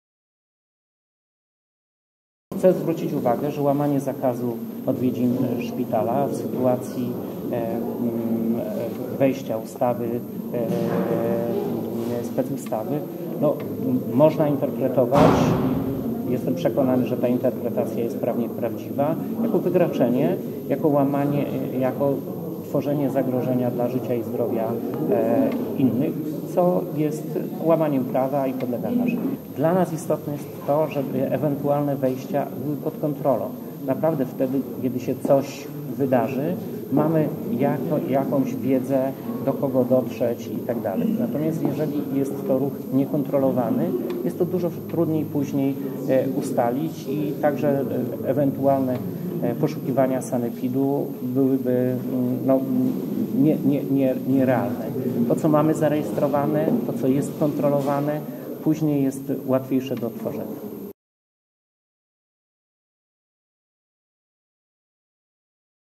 Będą kary za łamanie zakazu odwiedzin w szpitalach
Wojewoda dolnośląski zapowiada, że osoby łamiące zakaz odwiedzin w szpitalach będą musiały liczyć się z karami finansowymi. Wielu mieszkańców naszego regionu ignoruje środki bezpieczeństwa wprowadzone w związku z rozprzestrzeniającą się epidemią koronawirusa z Chin.